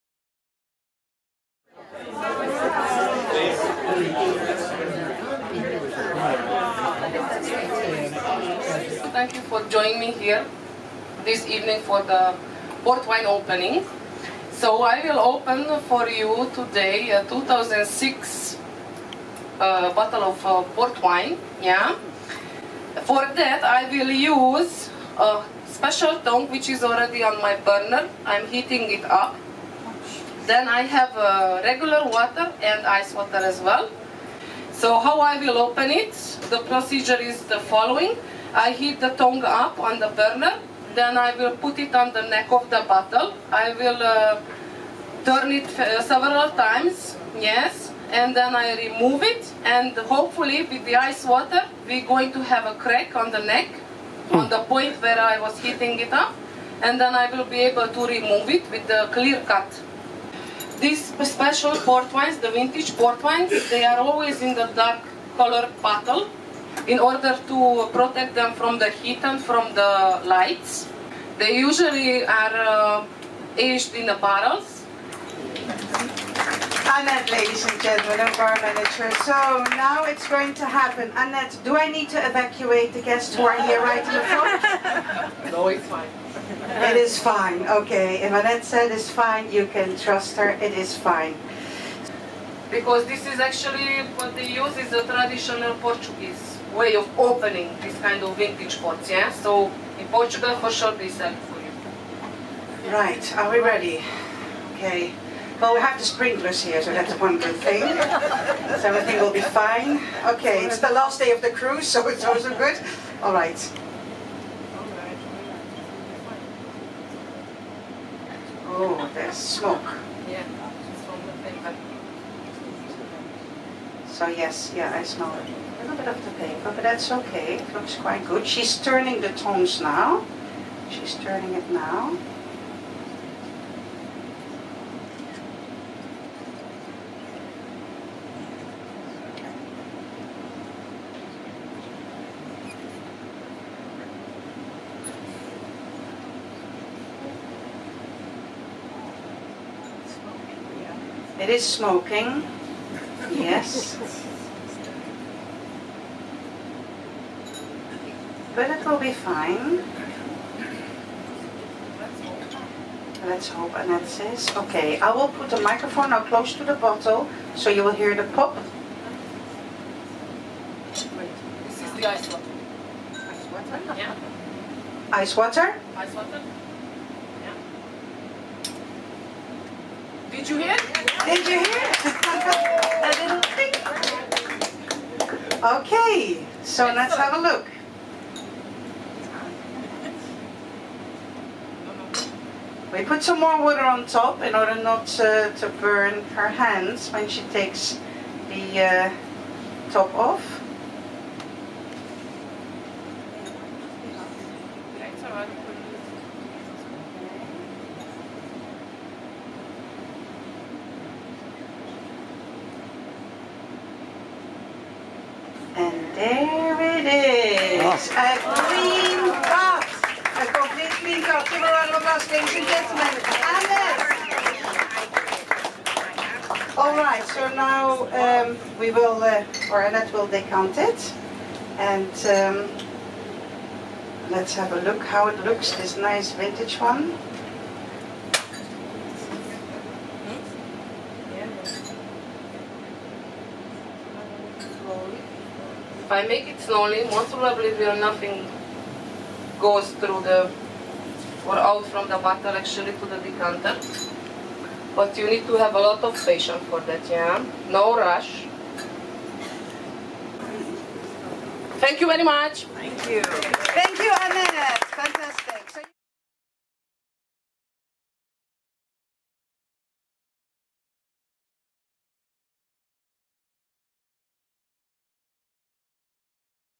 Traditional Port Wine Opening Demonstration
A demo given aboard a Viking river cruise.